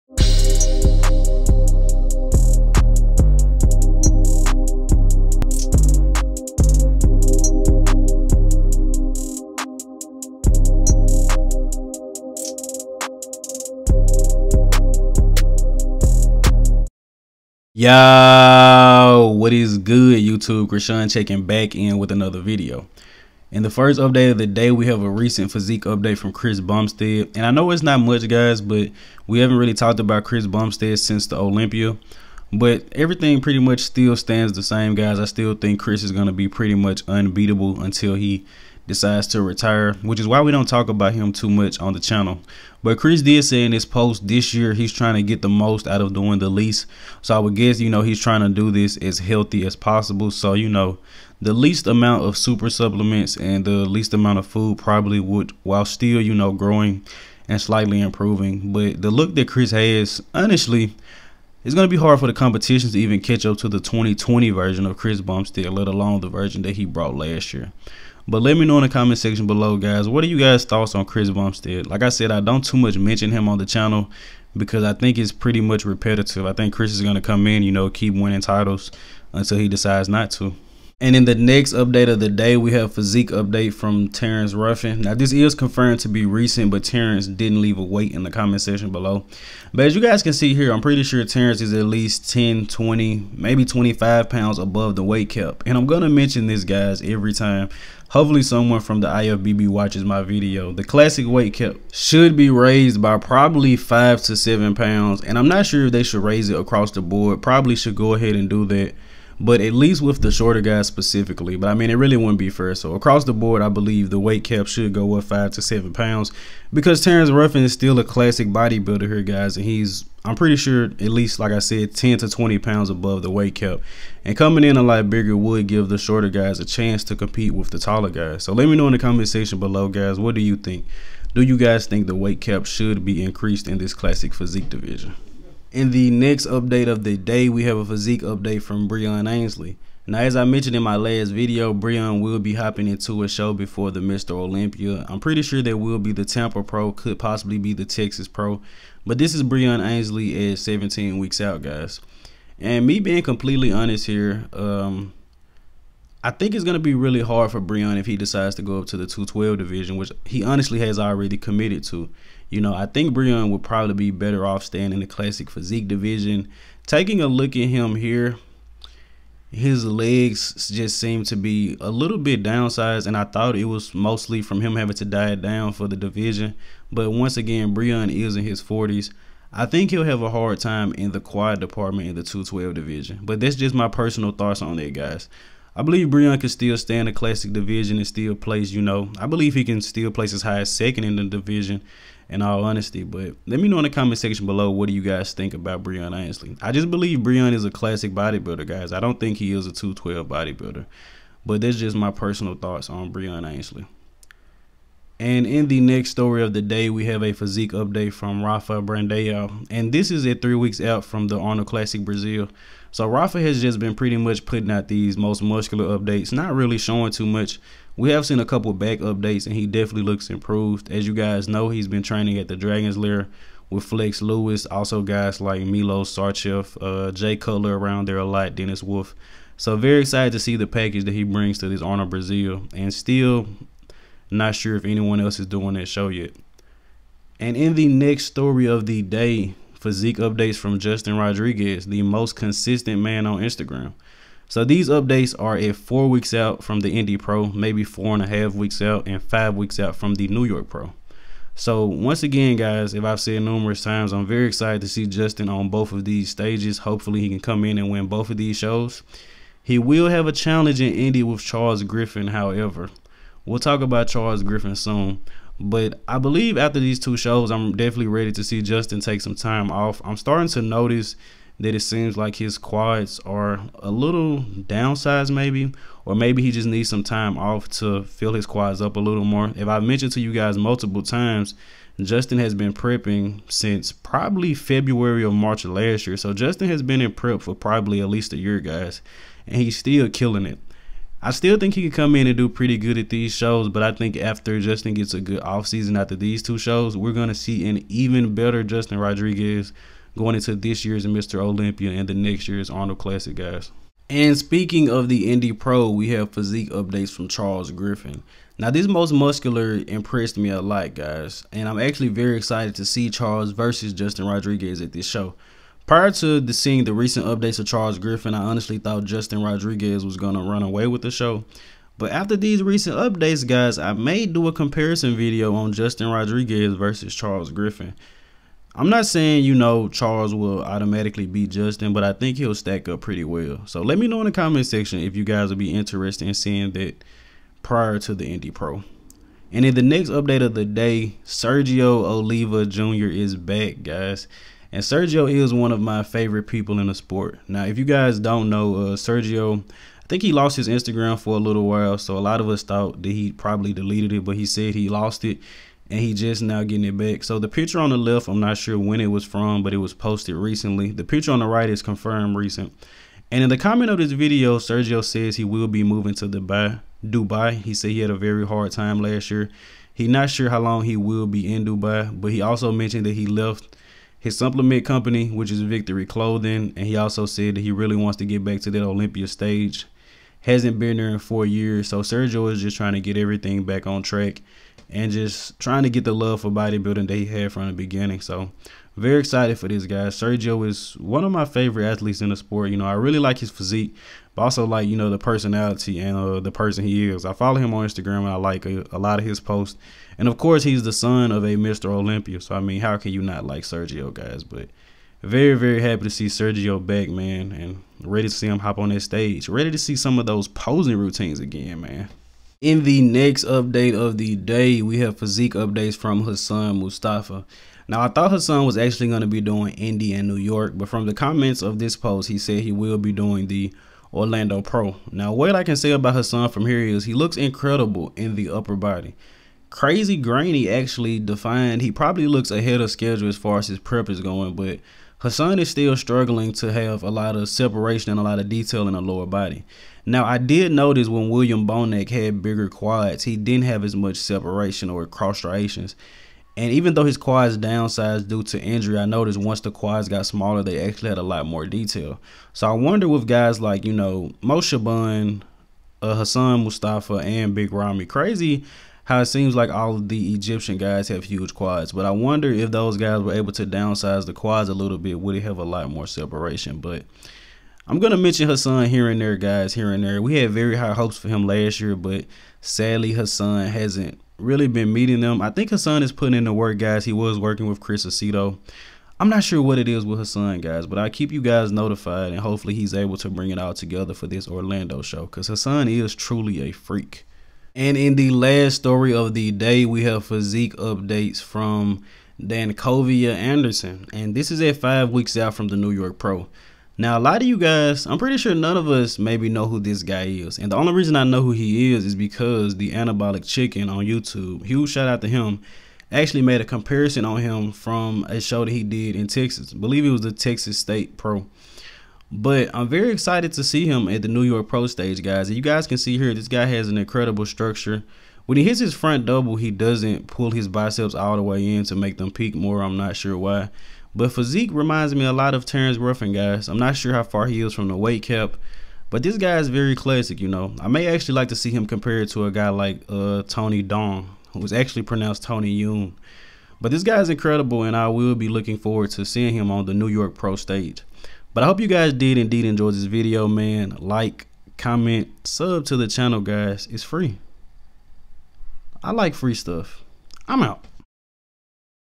Chris Bumstead Recent Update + Sergio Oliva Jr. is Back + Justin Rodriguez 4 Weeks Out + MORE
In this video I post updates of Chris Bumstead, Sergio Oliva Jr. , Justin Rodriguez, Charles Griffen, Breon Ansley, Terrence Ruffin, Dancovea Anderson + More.

Personal Instagram : @classicthegoat